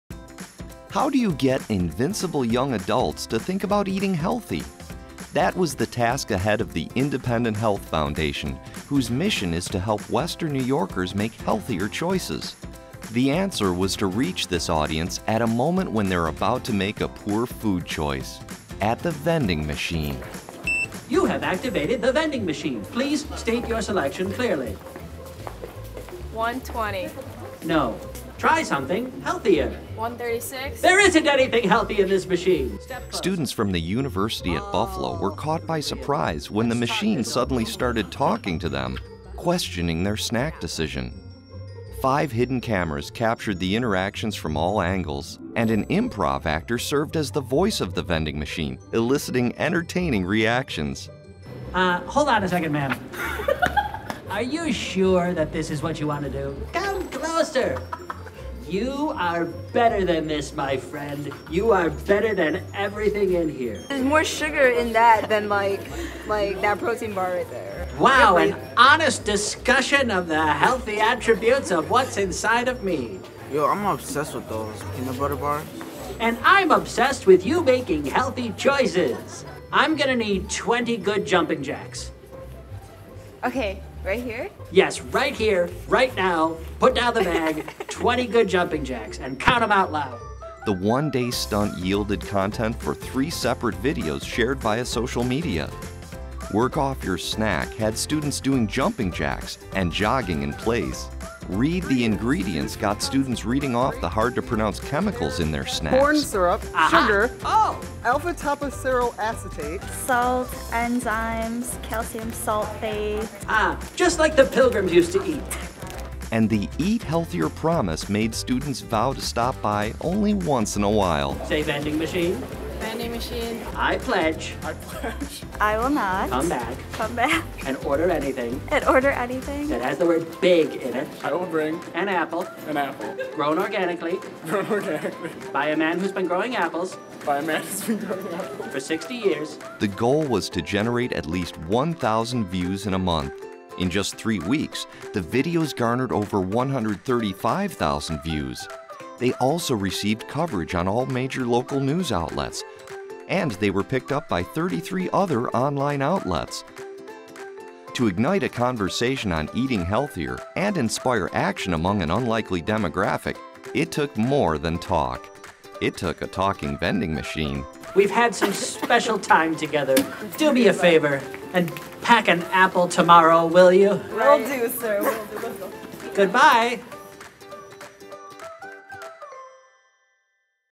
Talking Vending Machine Case Study
Who could forget the prank we pulled on these UB students that went viral?